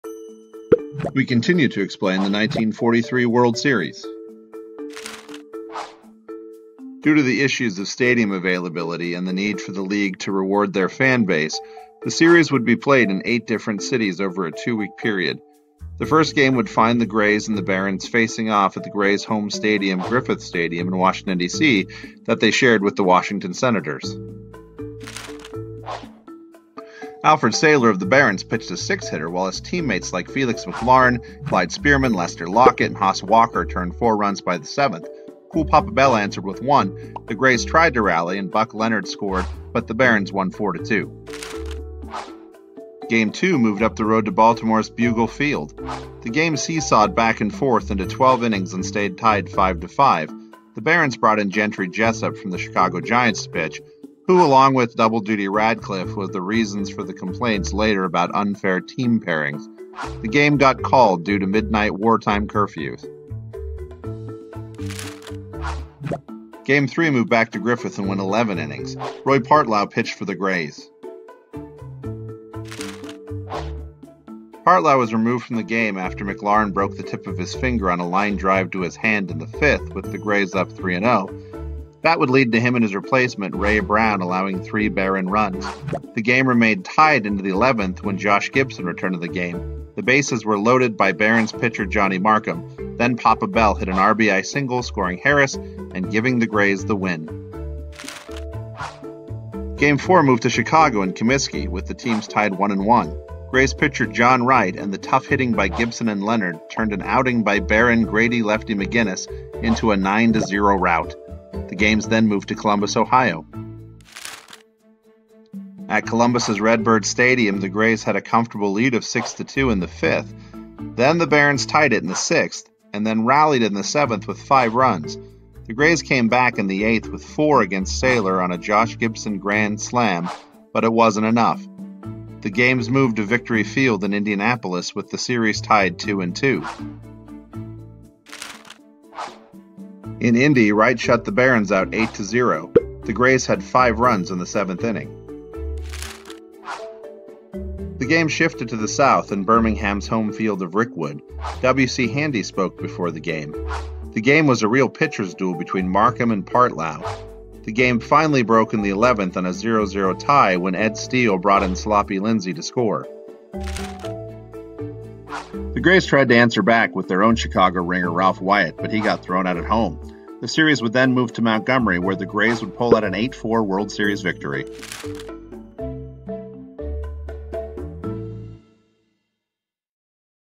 1943 World Series B -History of Baseball Class (Grays vs Black Barons)
Information collected from The Baltimore Afro-American, The Pittsburgh Courier, The Indianapolis Recorder, The Beckley Post-Herald, The Negro Leagues Museum, Wikipedia. Images not used with permission but with our apologies and hope for forgiveness, as we are a school project and make 0 financial gain from our product.